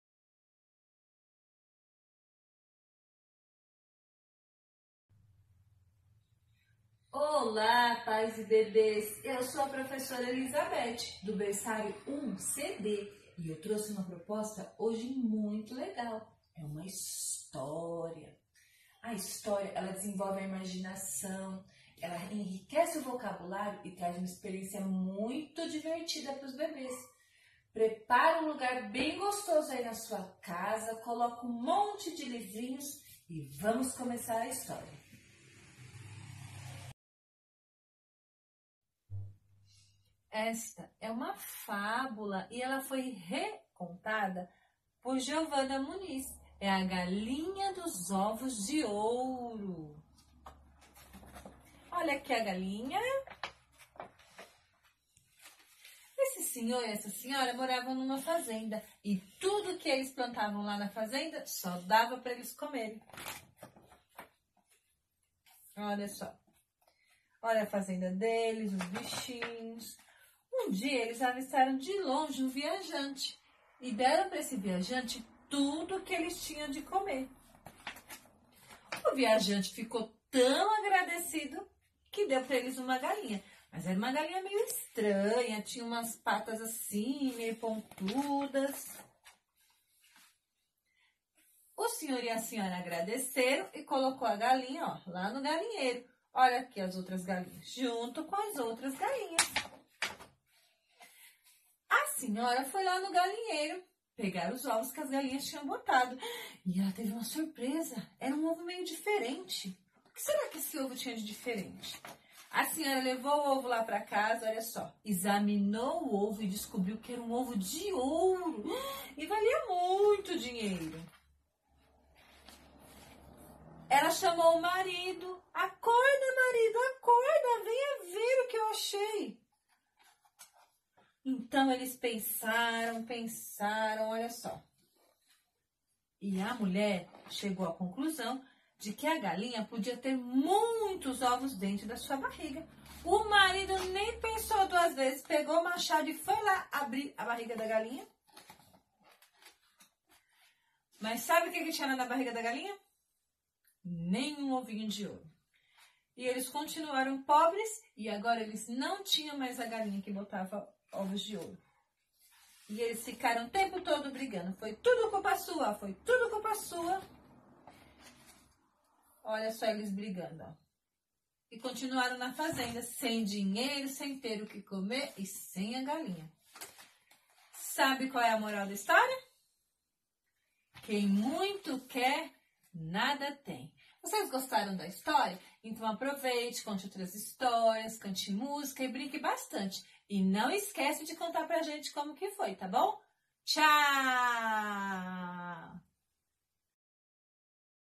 Atividade: Cantinho da História - CEI Jardim Arpoador
Vídeo de caráter educativo, produzido pela professora Elisabete de Sousa Santos do CEI Jardim Arpoador para os bebês do Berçário I A/B/C/D como forma de atividade durante o período de isolamento social.